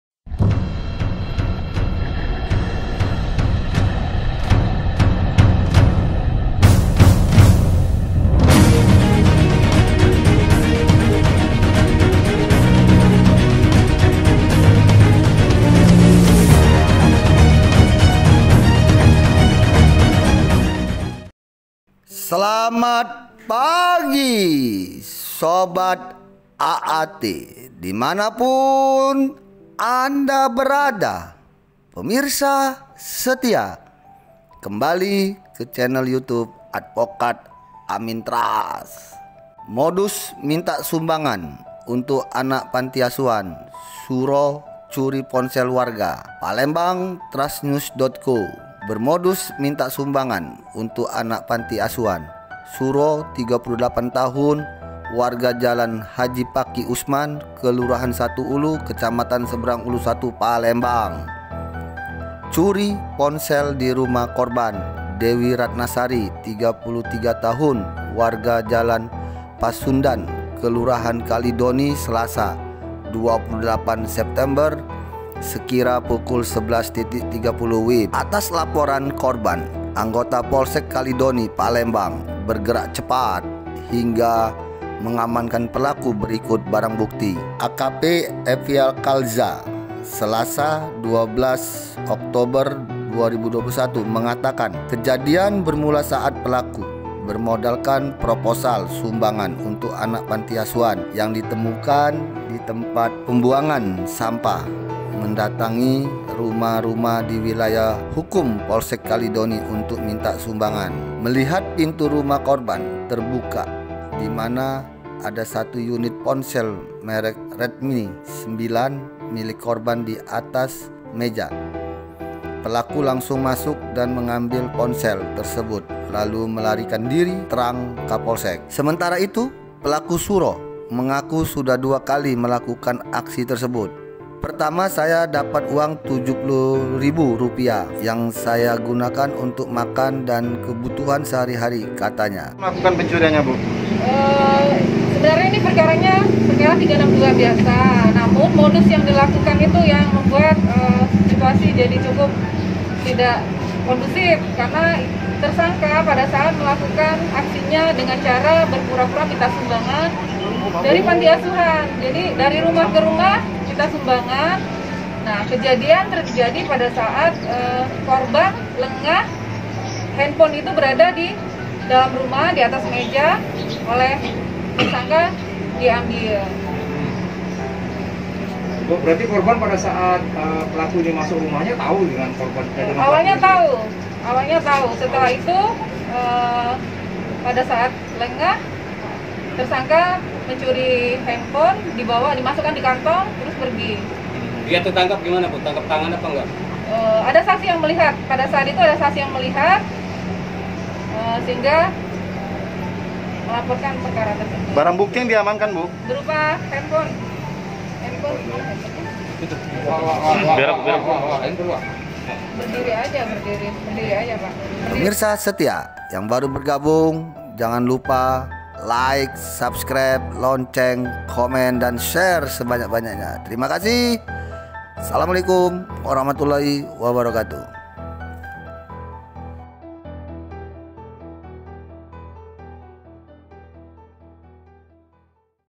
Modus Minta Sumbangan untuk Anak Panti Asuhan, Suro Curi Ponsel Warga ||COVID 19 WASPADA
Modus Minta Sumbangan untuk Anak Panti Asuhan, Suro Curi Ponsel Warga

PALEMBANG, Trasnews.co -

Bermodus minta sumbangan untuk anak panti asuhan, Suro (38) warga Jalan H Faqih Usman, Kelurahan 1 Ulu, Kecamatan Seberang Ulu  I Palembang curi ponsel di  rumah korban Dewi Ratna Sari (33) warga Jalan Pasundan, Kelurahan Kalidoni, Selasa (28/9) sekira pukul 11.30 WIB.

Atas laporan korban anggota Polsek Kalidoni Palembang bergerak cepat, hingga mengamankan pelaku berikut barang bukti.

Kepolsek Kalidoni AKP Evial Kalza, Selasa (12/10) mengatakan, kejadian bermula saat Pelaku bermodalkan proposal sumbangan untuk anak panti asuhan yang ditemukan di tempat pembuangan sampah, mendatangi rumah-rumah di wilayah hukum Polsek Kalidoni untuk minta sumbangan.

"Melihat pintu rumah korban terbuka dimana ada satu unit ponsel merek Redmi 9 milik korban di atas meja, pelaku langsung masuk dan mengambil ponsel tersebut lalu melarikan diri," terang Kapolsek.

Sementara itu, pelaku Suro mengaku sudah dua kali melakukan aksi tersebut.

'Pertama saya dapat uang Rp 70 ribu yang saya gunakan untuk makan dan kebutuhan sehari-hari," katanya. (AAT)